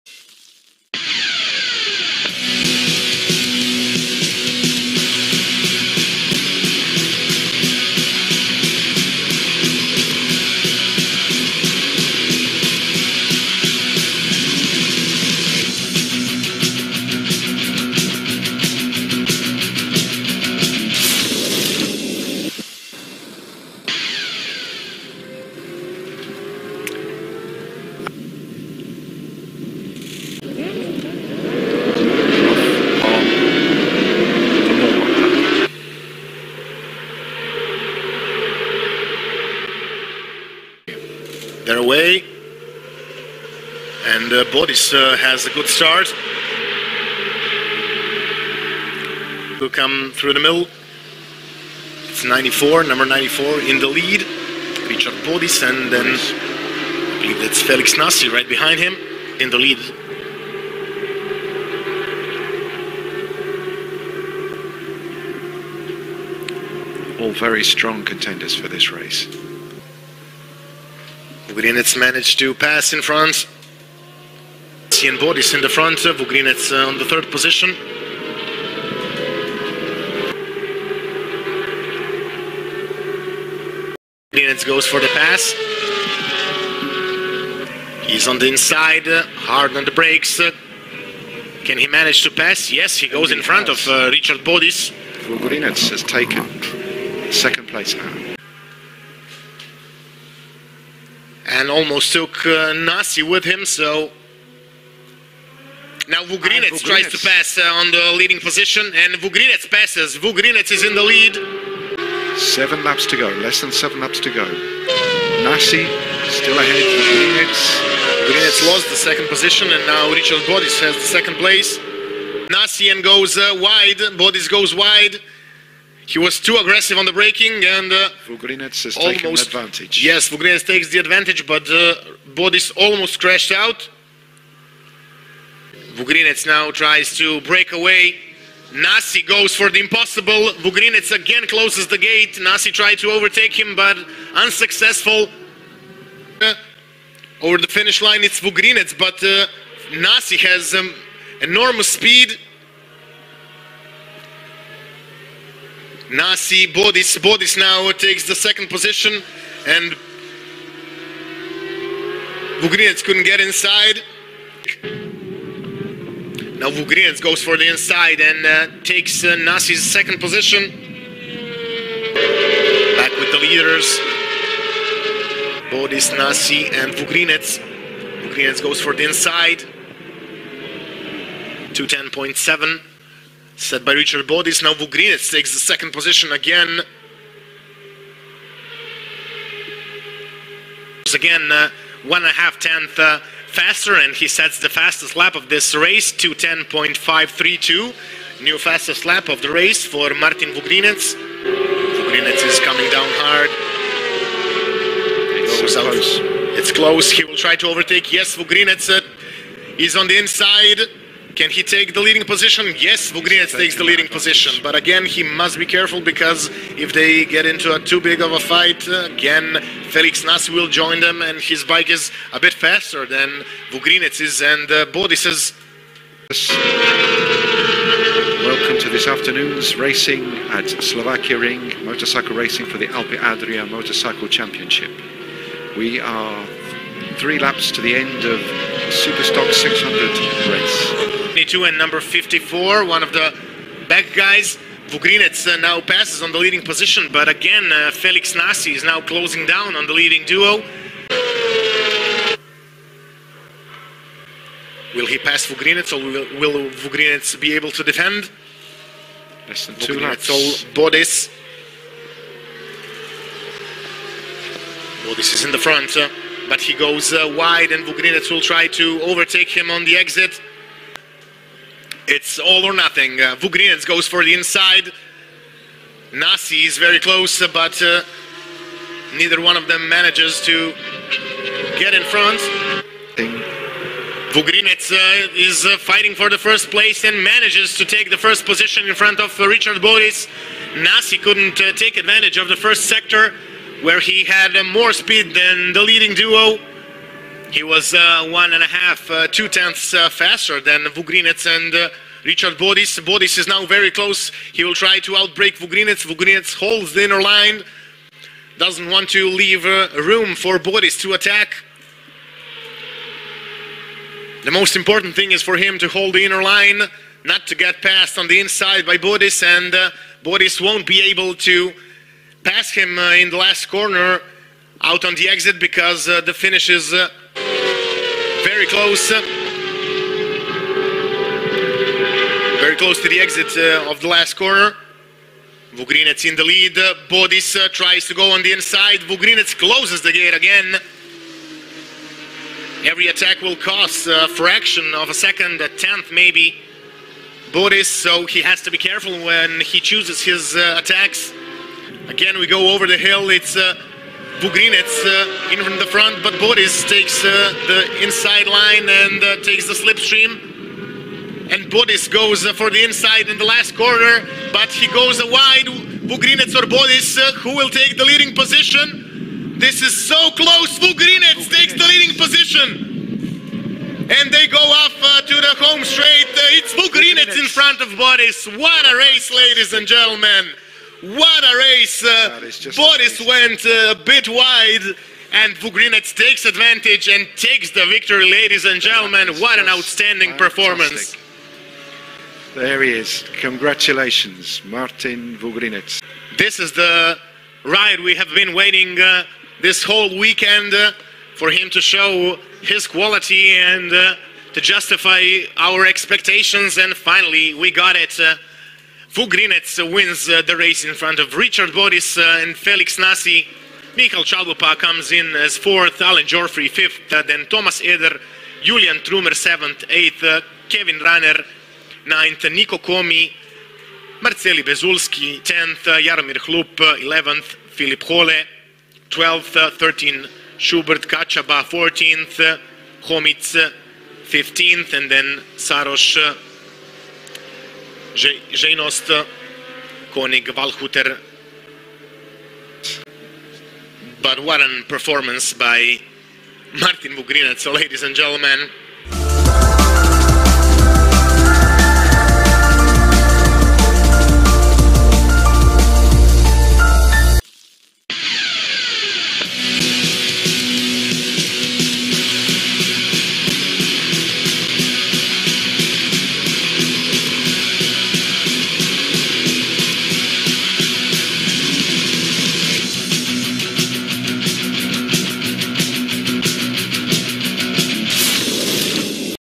Incredible Race✅Martin Vugrinec 2019 ALPE ADRIA Slovakiaring / STK 600 /dp tuning /174. MOTO 30+
Incredible Race 2 !  Martin Vugrinec - SPIDERMARTIN
STK 600 - Slovakiaring 2019. / best moments/
 Alpe Adria International Motorcycle Championship
Must See Last Lap!